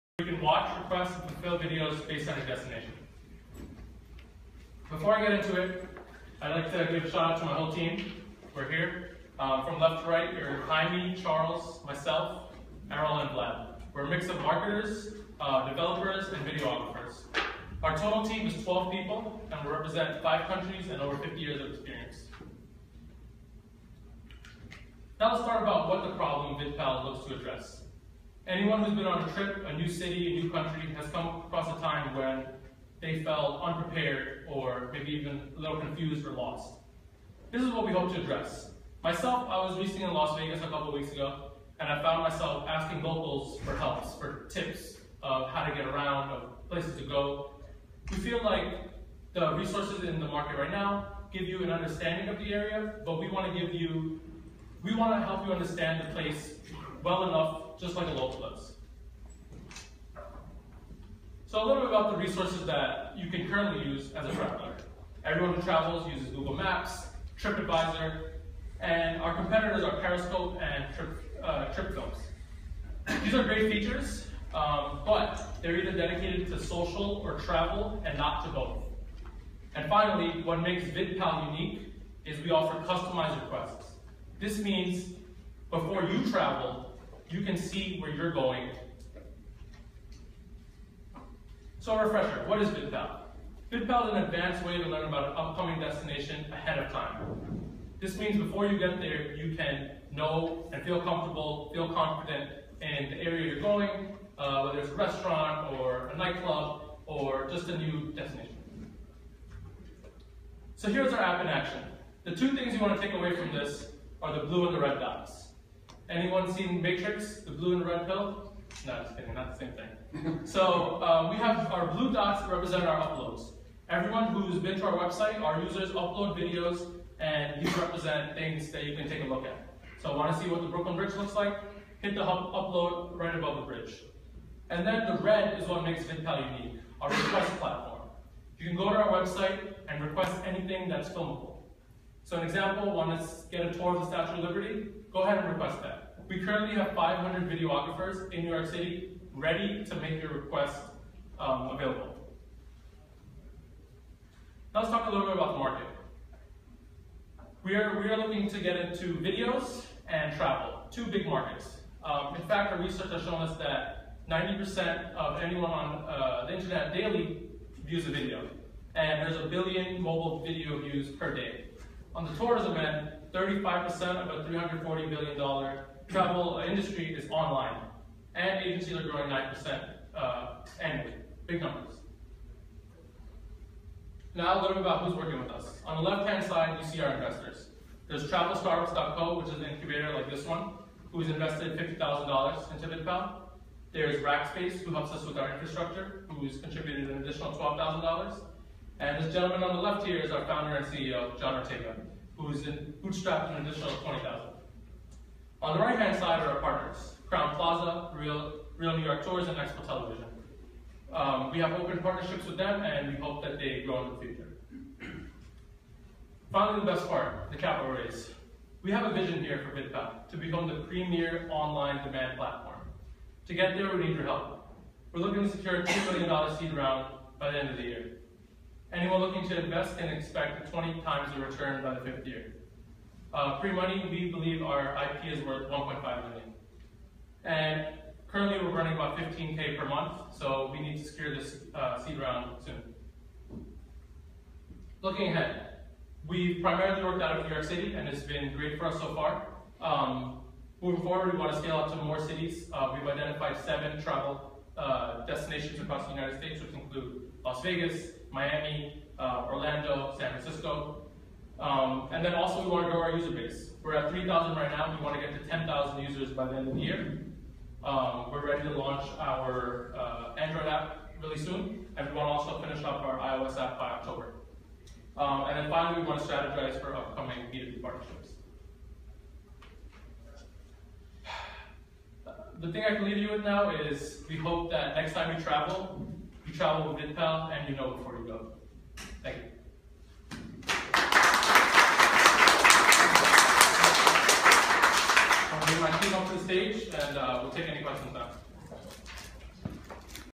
Vidpal Videos: knowbeforeyougo 143-151 varick st, new york, ny 10013, usa
The Vidpal team was out last night making it's pitch to peers and investors.